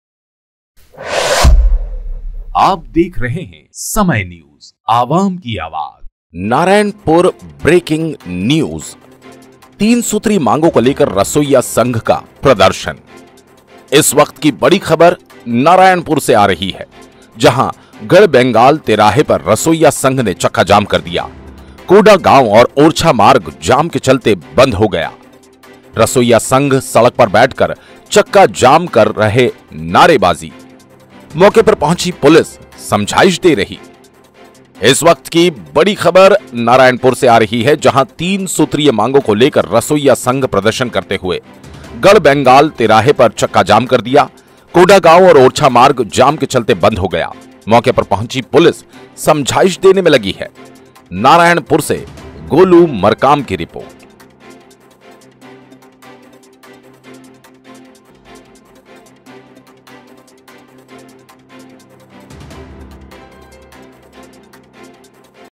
इस वक्त की बड़ी खबर नारायणपुर से आ रही है .. 3 सूत्री मांगों को लेकर रसोईया संघ का प्रदर्शन
About this video - इस वक्त की बड़ी खबर नारायणपुर से आ रही है .. 3 सूत्री मांगों को लेकर रसोईया संघ का प्रदर्शन